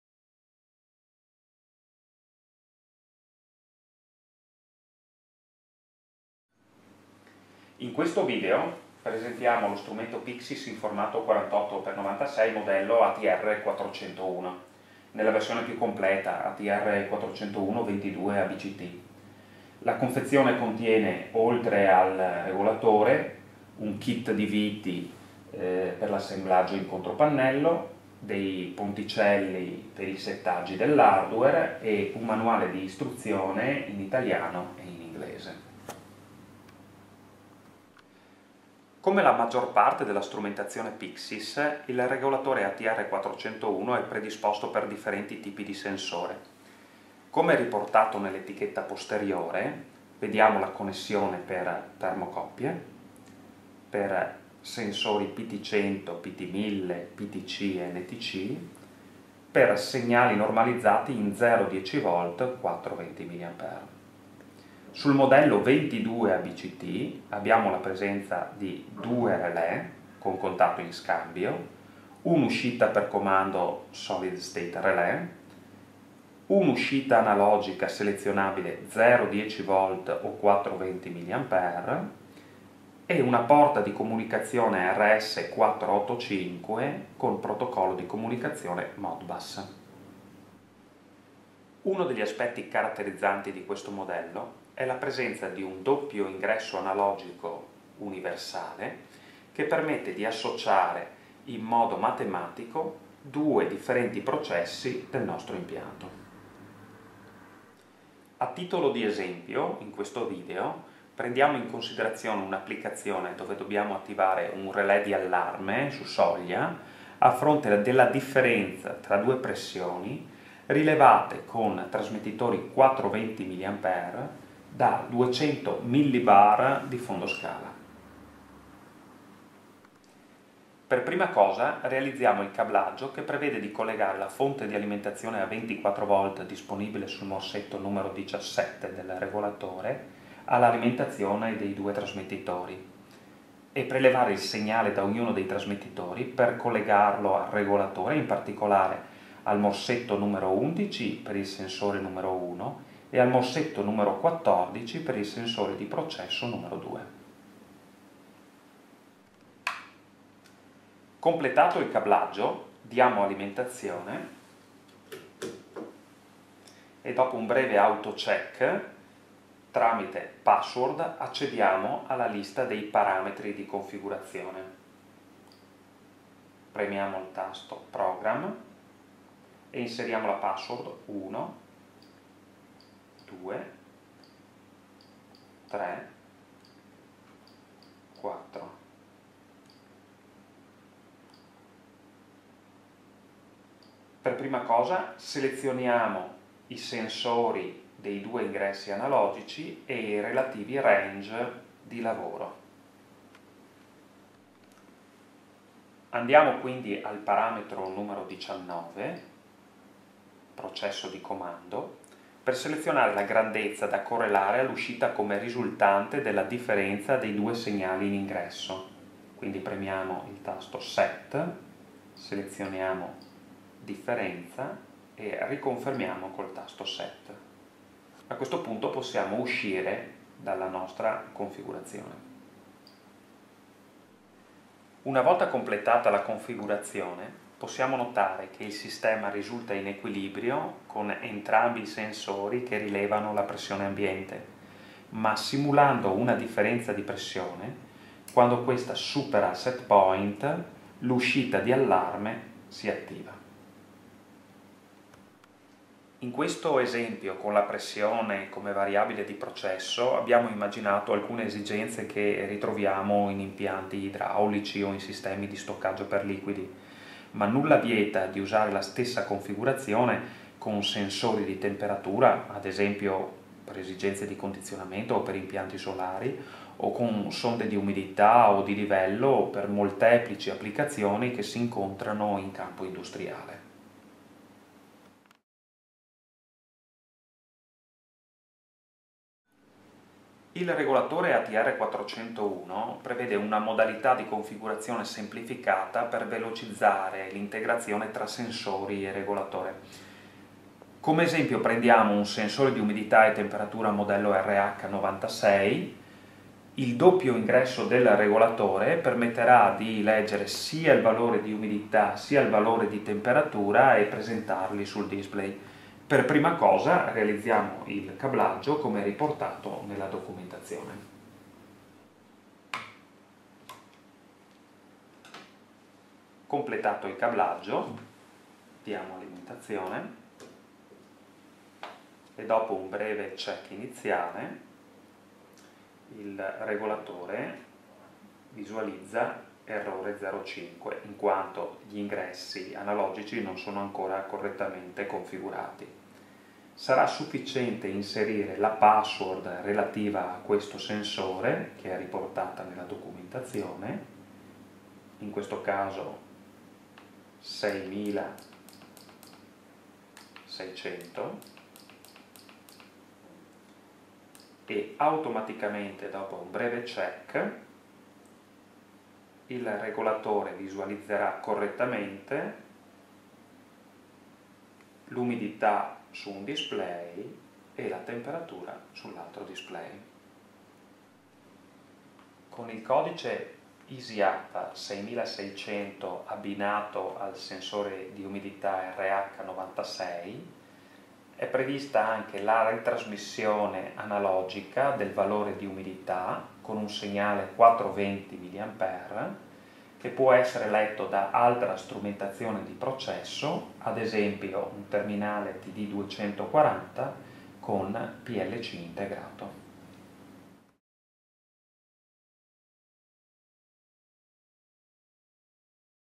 Regolatore industriale - PID controller ATR401 Pixsys [SUB ENG]
Regolatore ATR401 esempi applicativi con doppio ingresso analogico e configurazione veloce con codice EASY-UP.
48x96mm (1/8 DIN), Singolo/Doppio Loop di controllo, caratterizzato da elevata configurabilità in termini di ingressi e uscite.
Due ingressi analogici sono configurabili per una vasta gamma di sensori con la possibilità di regolare due processi distinti oppure di gestire operazioni matematiche (somma, differenza, media) tra i due processi. Uscite a relè / logiche / analogiche selezionabili come comando, allarme e ritrasmissione del dato in ingresso. Utile alimentazione a range esteso da 24 a 230V AC/DC con isolamento galvanico dalla rete, isolamento galvanico tra ingressi uscite e seriale di comunicazione RS485

Regulator ATR401, applications examples with dual analogue input and quick setup with EASY-UP code.
Controller 48x96mm (1/8 DIN), Single/Double Control loop, characterised by high configurability of the inputs and outputs.
The two analogue inputs can be configured for a vast range of sensors with the option of controlling two distinct processes or managing mathematical operations (sum, difference, average) between the two processes. Power supply range from 24 to 230V AC/DC. Outputs can be selected as control, alarm, analogue retransmission. Useful power supply range from 24 to 230V AC/DC with galvanic isolation from the network, galvanic isolation between inputs and outputs and isolated serial RS485.